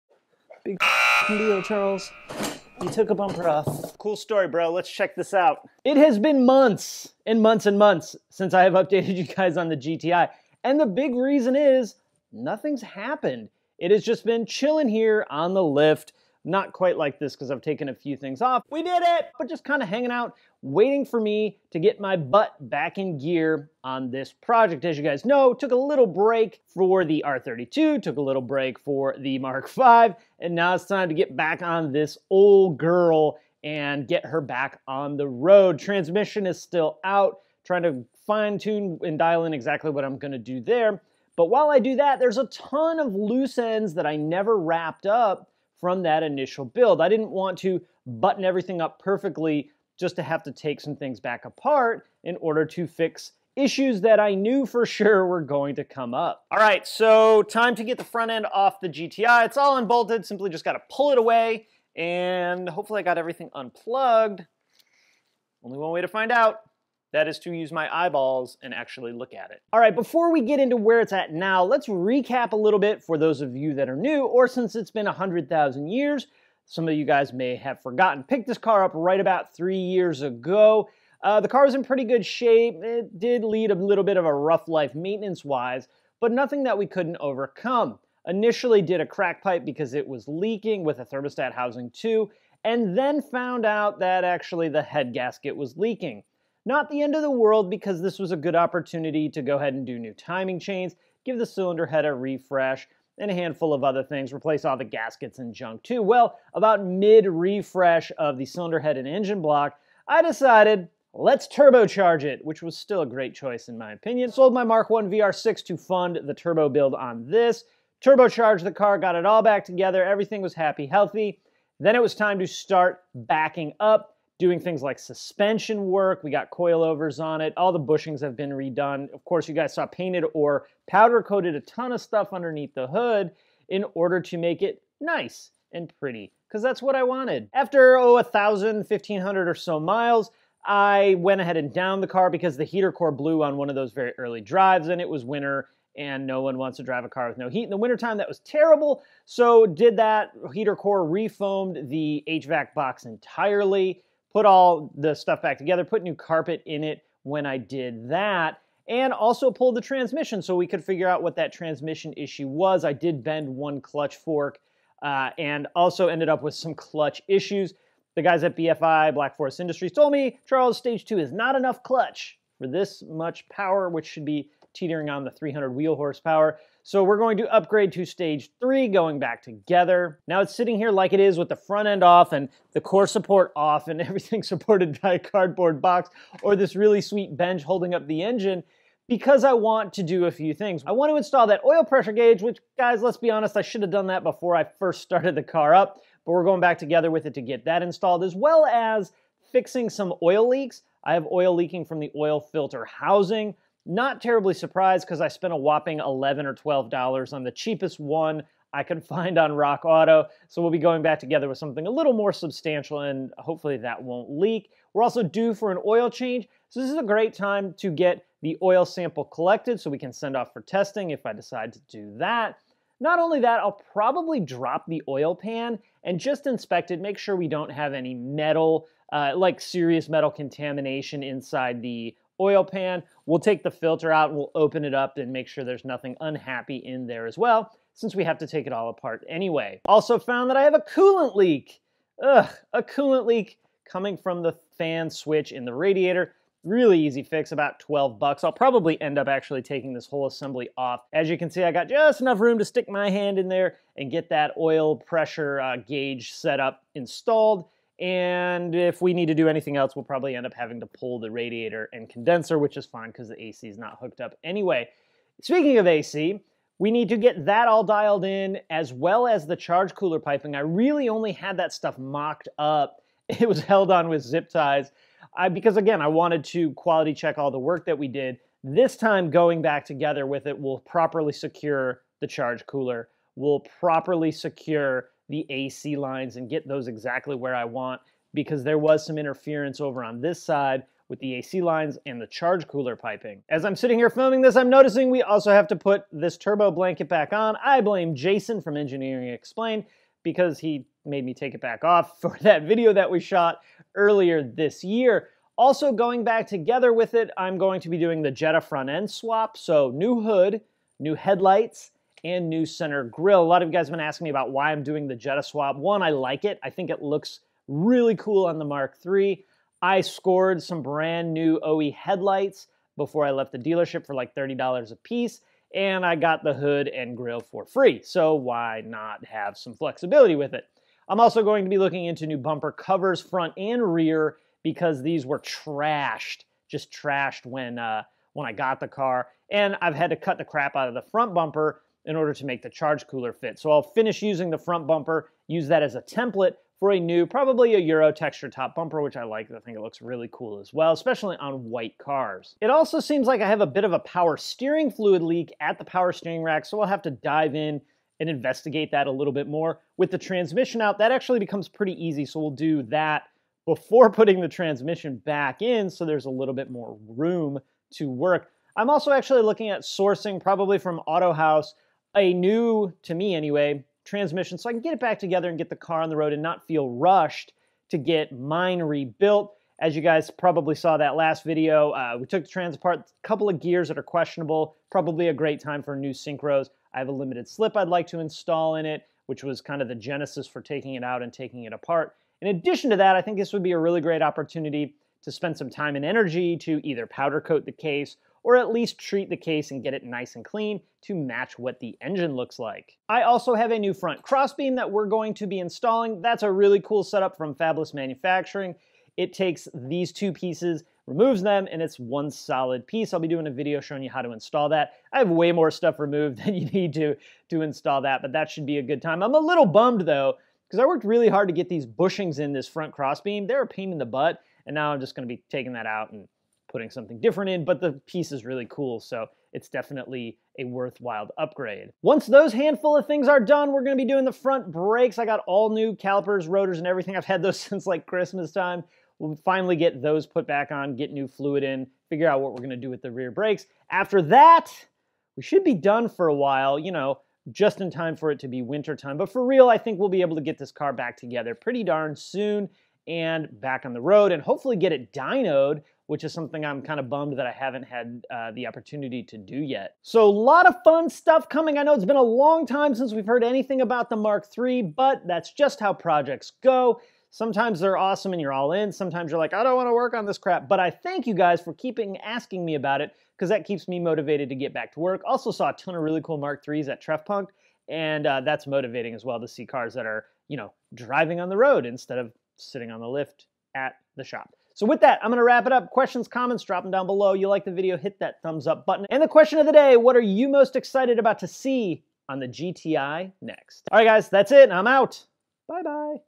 VR6t GTI Update ~ Stalled Project and Fluid Leaks
The VR6 turbo project was stalled. But now it's time to get back to the MK3. I have oil leaks to fix, coolant leaks to fix, charge cooler piping to  fix, a clutch to fix, a transmission to fix, some more parts to install, brakes to upgrade and probably a whole lot more. MK3 LIFE!!